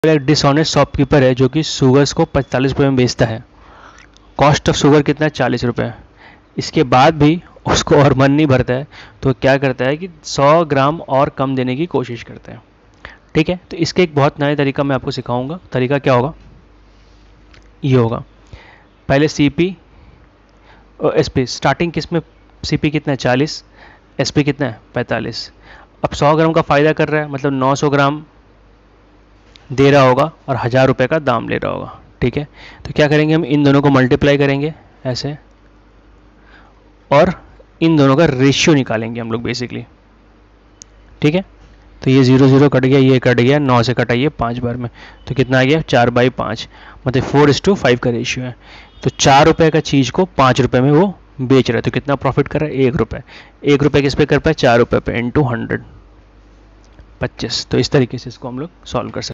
A dishonest shopkeeper sells sugar at a rate of Rs. 45 per kg
A dishonest shopkeeper sells sugar at a rate of Rs. 45 per kg. The cost price of the sugar is Rs. 40 per kg. Even after doing this, he tries to increase his profit by removing 100 gm from each packet of 1 kg. What is the gain percent of the shopkeeper?एक बेईमान दुकानदार चीनी को 45 रुपये प्रति किलोग्राम की दर से बेचता है। चीनी की लागत 40 रुपये प्रति किलोग्राम है। यह करने के बाद भी, वह प्रत्येक 1 किलोग्राम पैकेट से 100 ग्राम चीनी निकालकर अपना लाभ बढ़ाने की कोशिश करता है। दुकानदार का लाभ प्रतिशत क्या है?